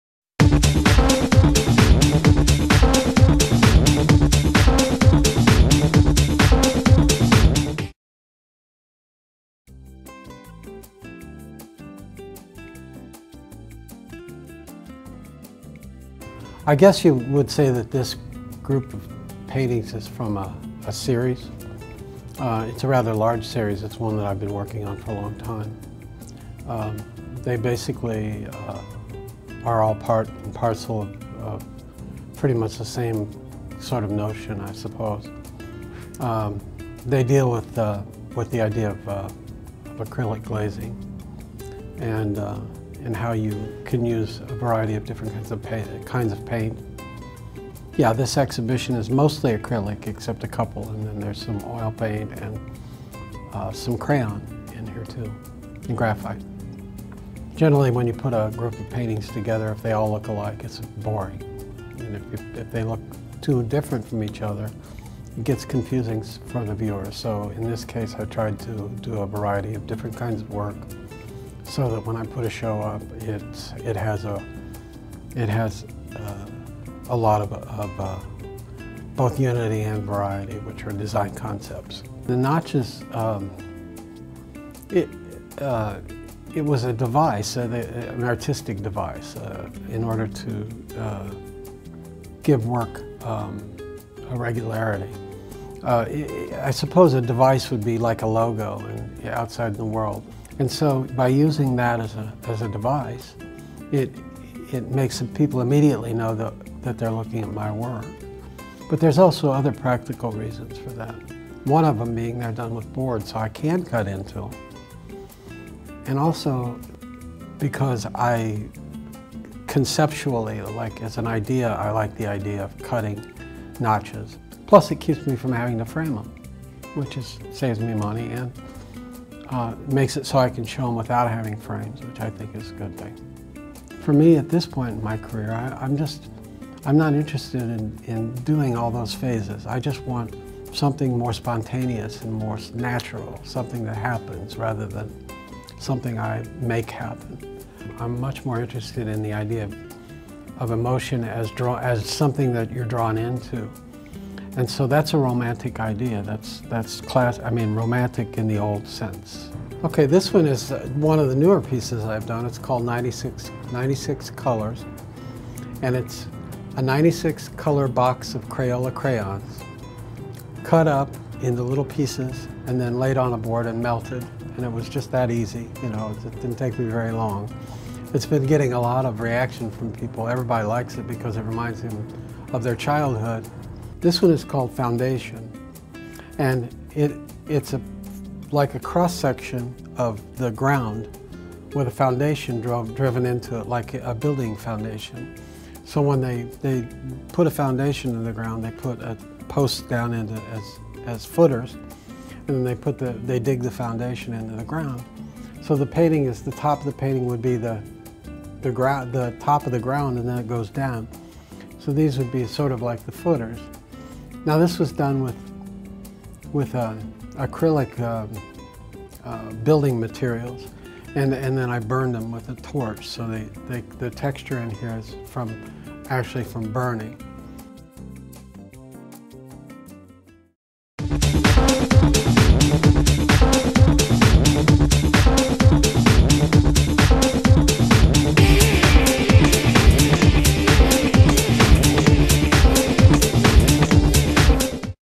SSC 360 - Steve Sherrell
In this "Artist Spotlight", local artist Steve Sherrell came to South Suburban College to talk about his exhibition entitled "Pure".